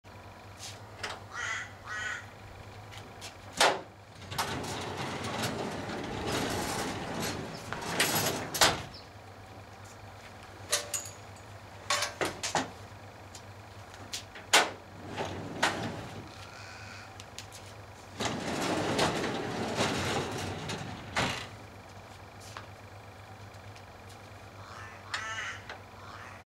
Scrubbers Camper Trailers luggage bay
Store all you camping items so you do not have to climb in back of trailer.